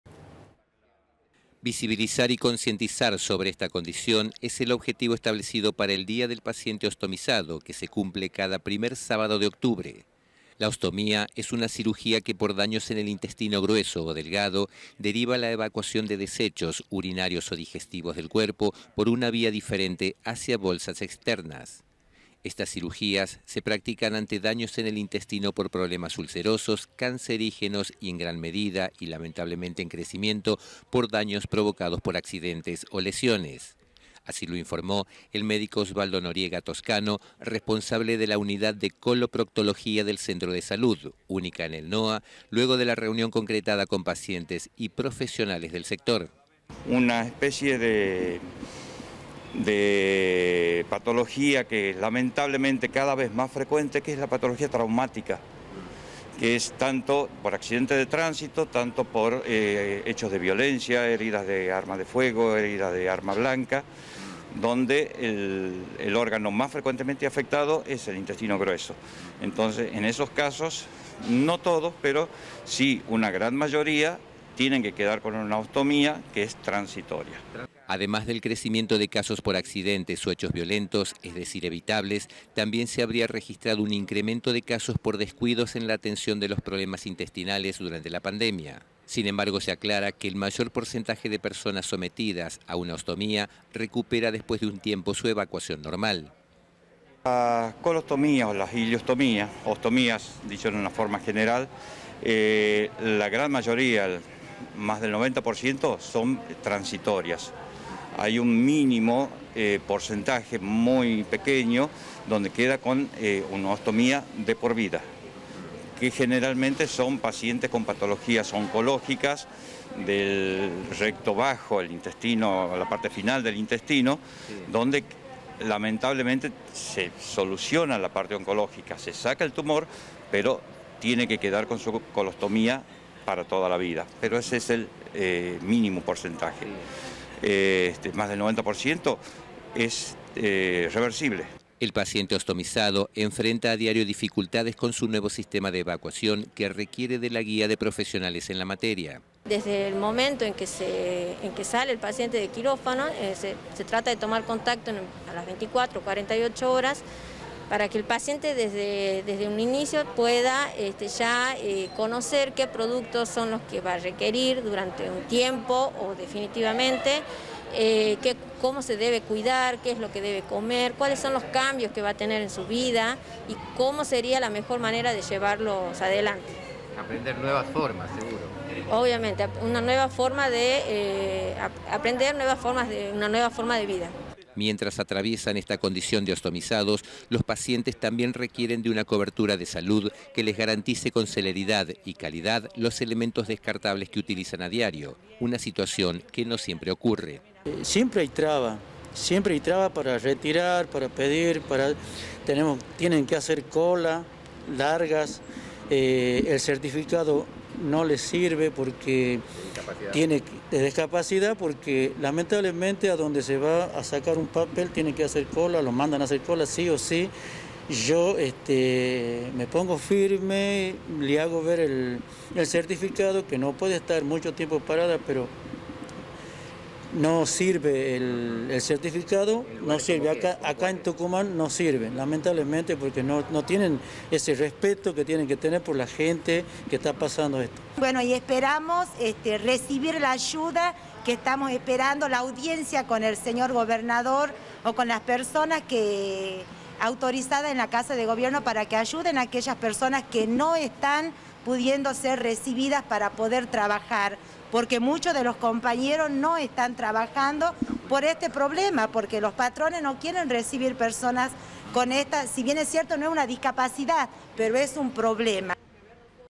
Celebran el Día del paciente Ostomizado
Cada primer sábado del mes de octubre se celebra el día del paciente ostomizado. Un paciente ostomizado es aquel que por un cáncer, ulcera o un accidente en la zona intestinal -generalmente- tuvo que ser sometido a una cirugía que le permita evacuar sus desechos corporales en una bolsa de colostomia externa.

Esta condición no es permanente en la mayoría de los casos, pero si evitable. Según profesionales de esta especialidad la población debe prestar atención a determinados cuidados para evitar esta situación. Paralelamente los pacientes también tienen algunas dificultades para garantizar los insumos descartables que necesitan.